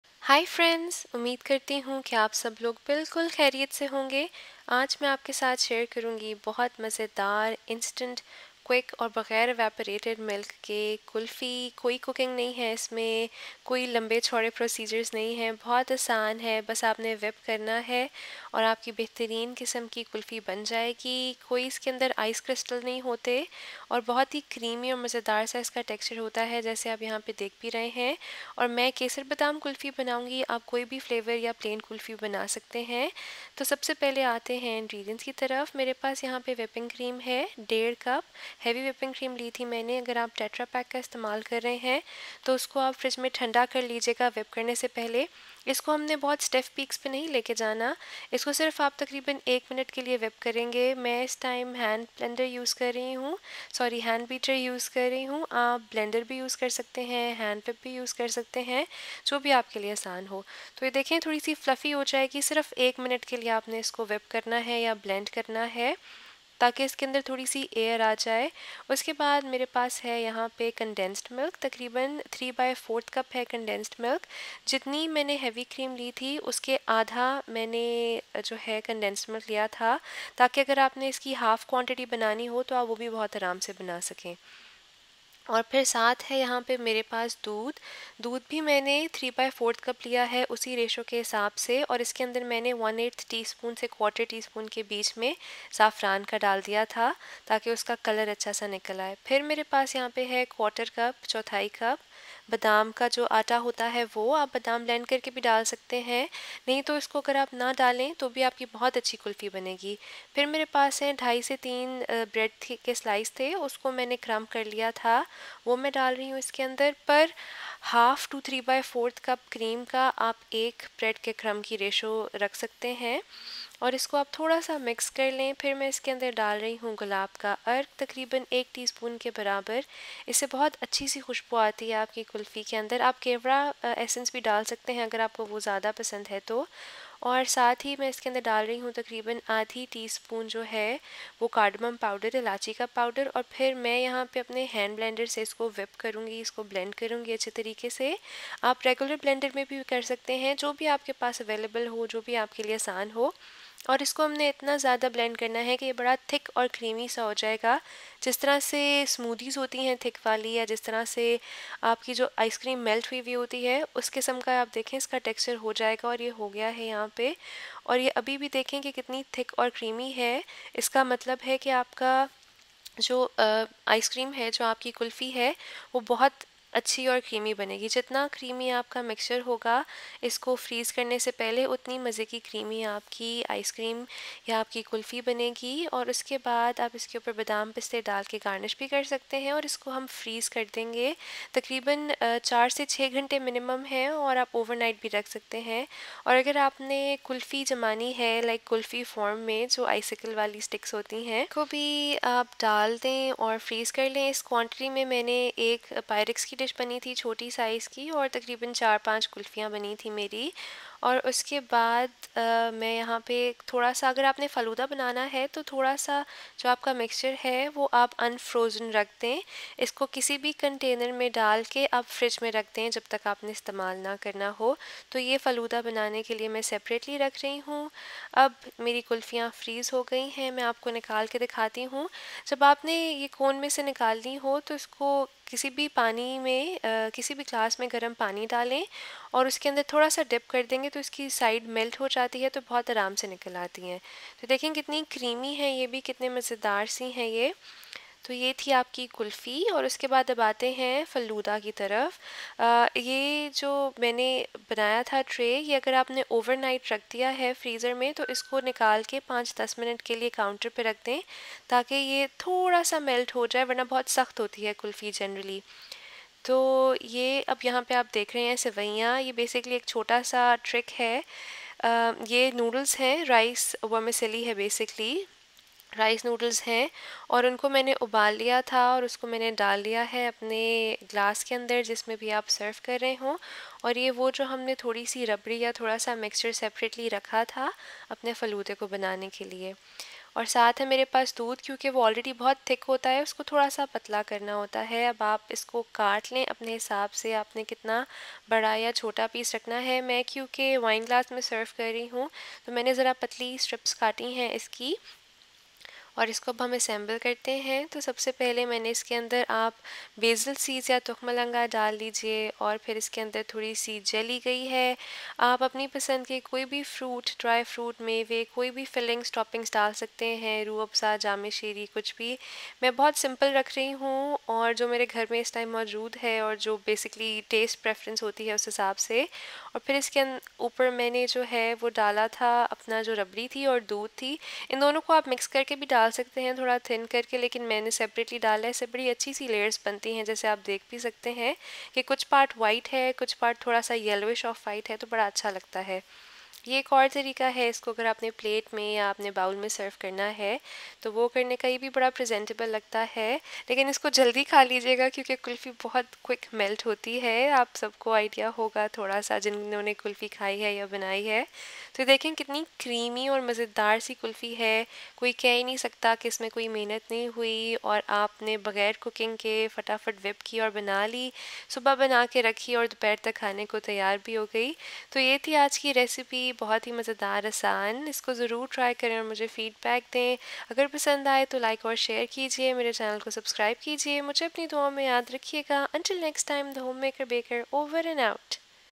Instant Kulfi Falooda Recipe without Evaporated Milk | No Cook Kulfi with Condensed Milk
Easy and instant no cook Kulfi & falooda that is ready to be served within a few hours. Few ingredients & a blender/processor is all that is needed for this super creamy kulfi.  It has no ice crystals and most of all it can make any flavour like I made Kesar Badam Kulfi. Use the base & flavour it with anything of choice such as mangos to make Mango Kulfi.